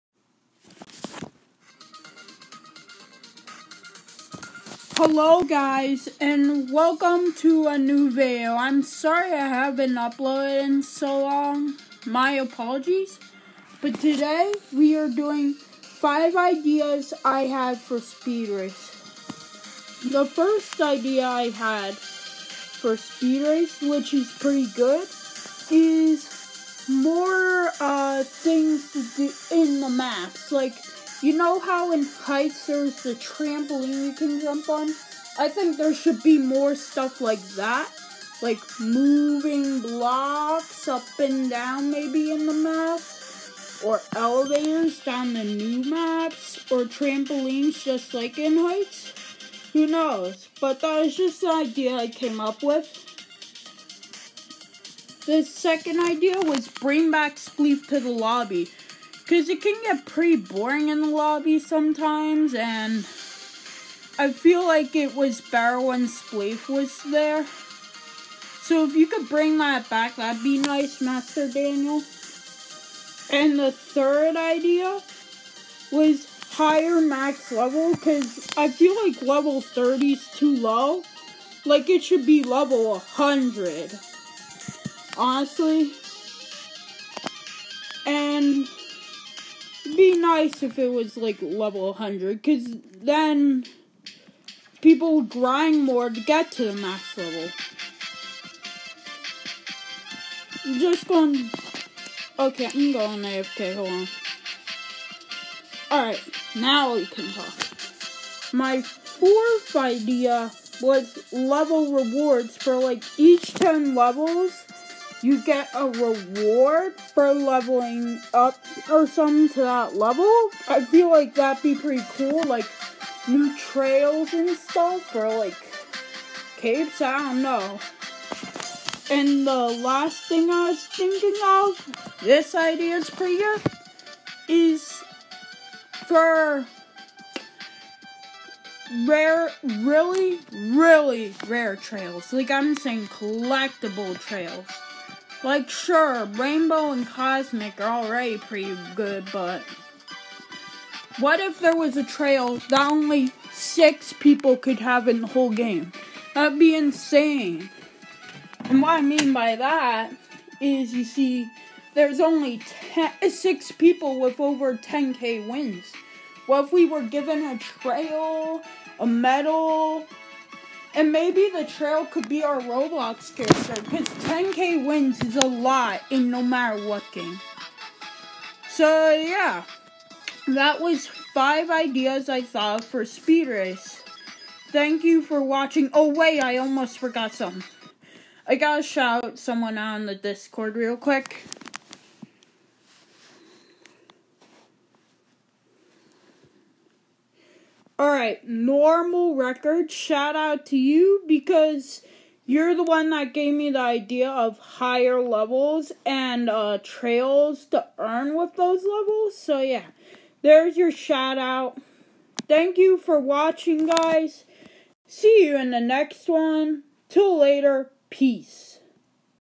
5 Great Ideas I Thought Of For Speed Race
Today I say 5 ideas that I think would be good for speed race